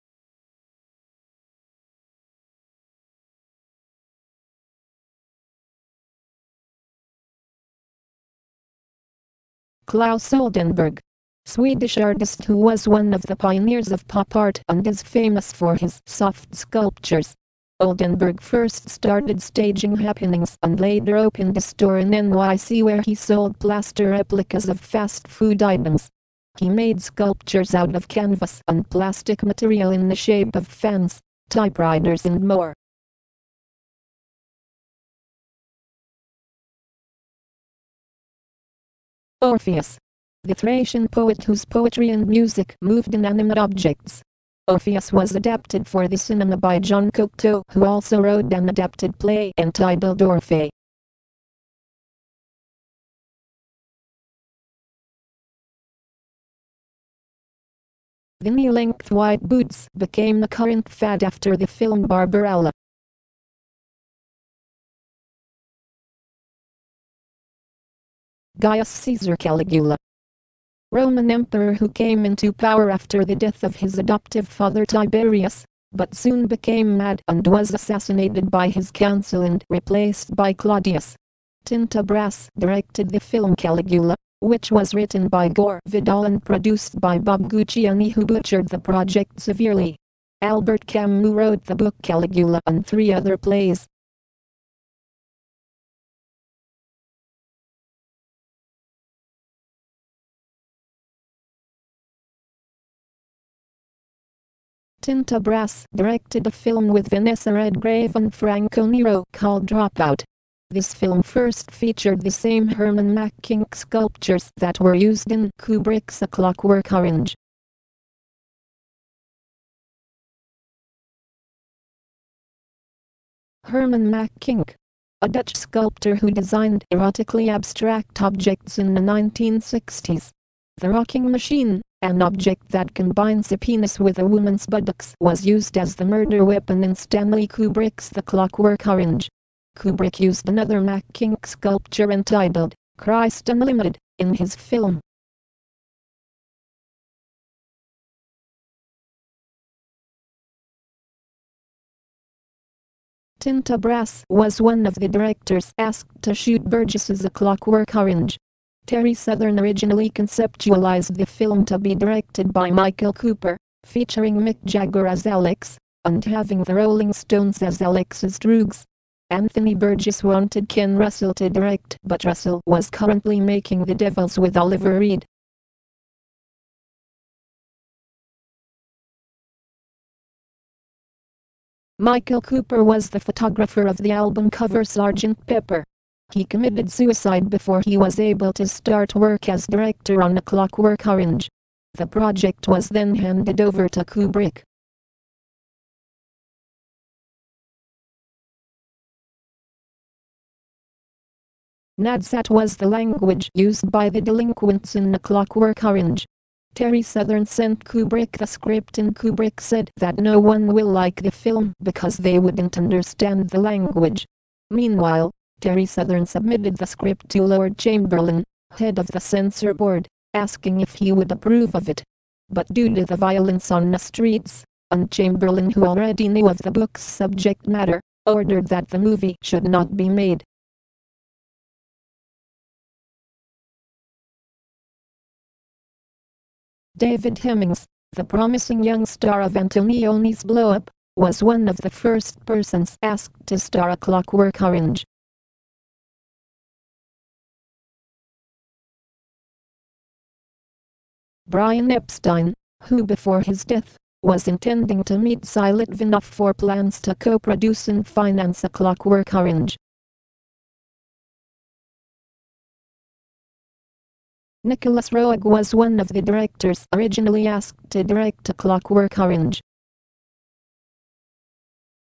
DV's Notebook Re-enacted
My notes set to moving images. Titles include, as follows: Claes Olderburg, Orpheus, White Boots, Caligula and The Clockwork Orange Bit.